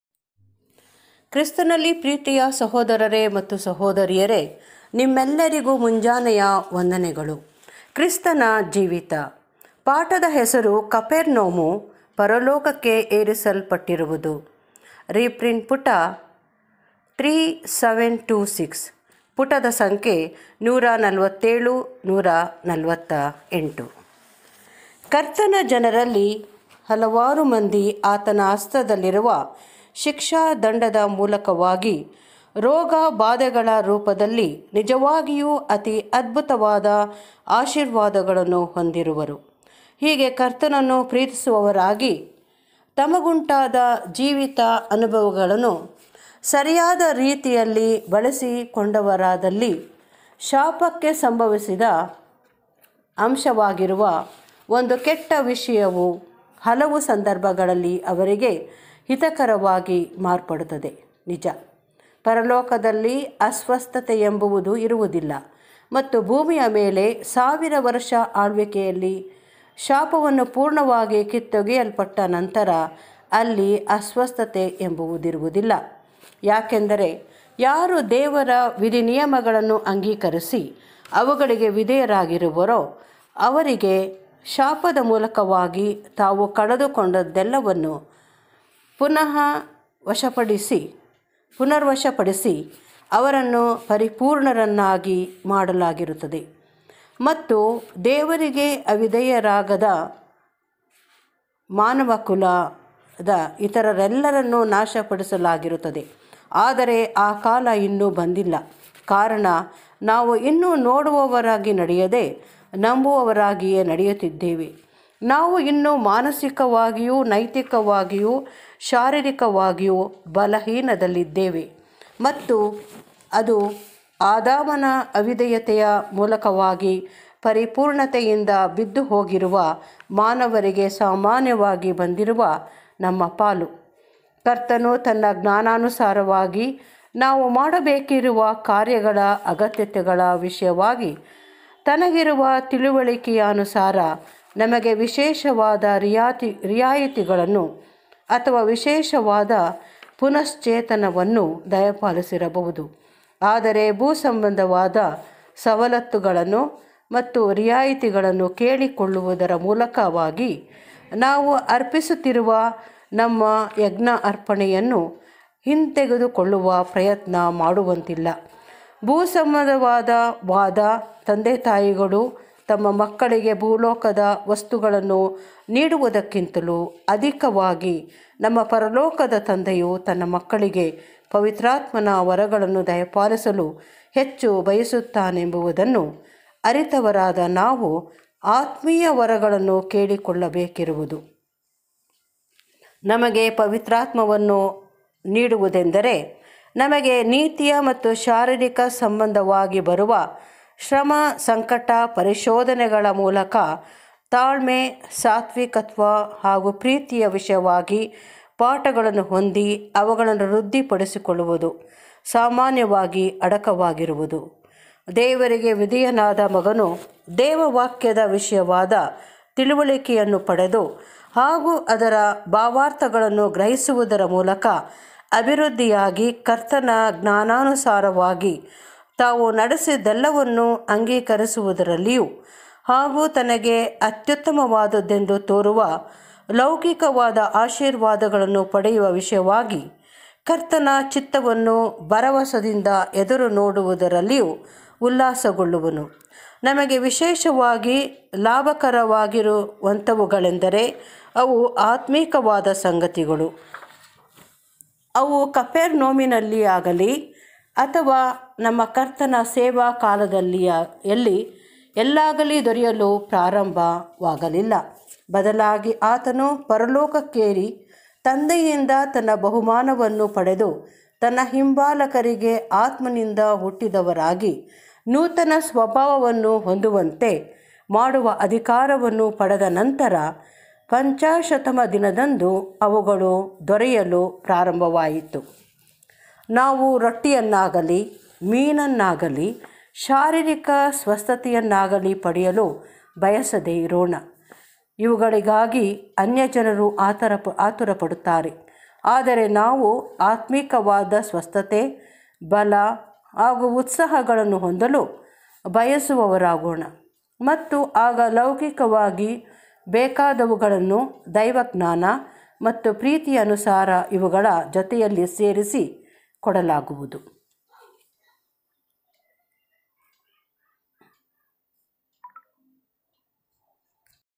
Audio (Kannada)Recording Life Of Christ Capernaum, which art exalted unto heaven R3726 -5, Pg146-147
Audio (Kannada) 📚 Recording Life Of Christ 📖 Capernaum, which art exalted unto heaven R3726 - Part-5, Pg 146-147 Free Bible Study Br Raju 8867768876 -  Jesus Loves Ministry